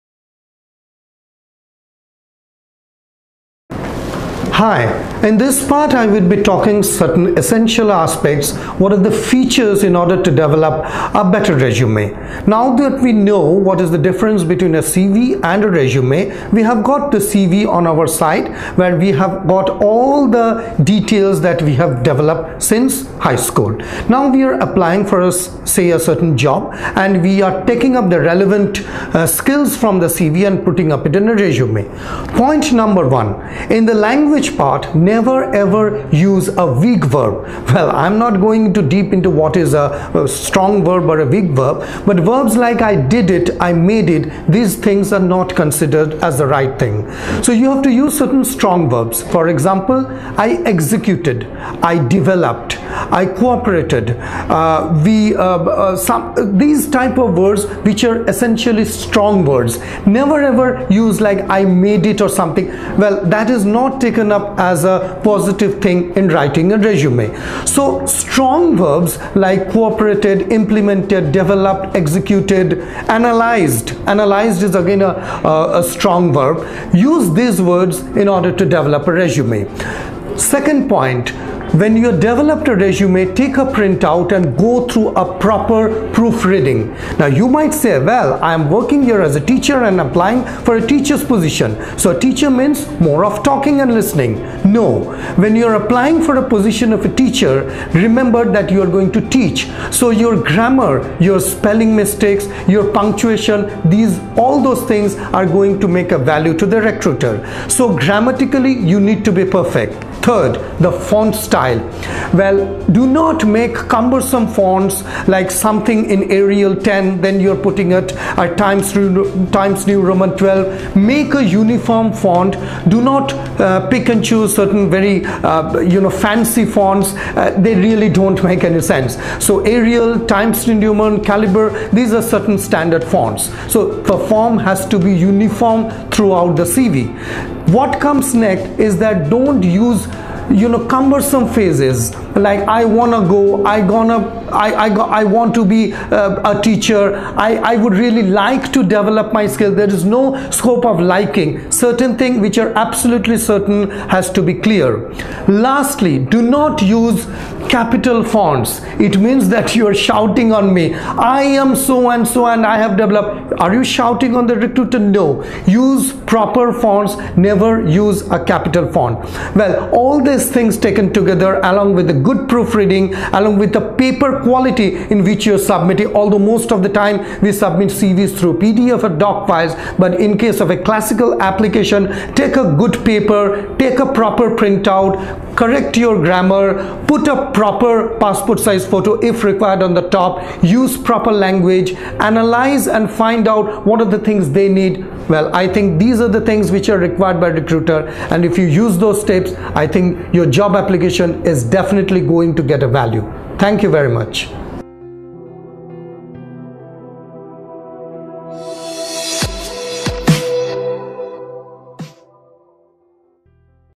What are the features to develop better resume
Often times candidates miss out on the essential part of making a resume. If you have missed the earlier video on the difference between a CV and a Resume, please watch out that first.

This video explains what type of language, font, color, size and language you should use to build up an effective resume. Also, there should not be any error in punctuation, grammar and spelling as it would degrade your value in front of the recruiter.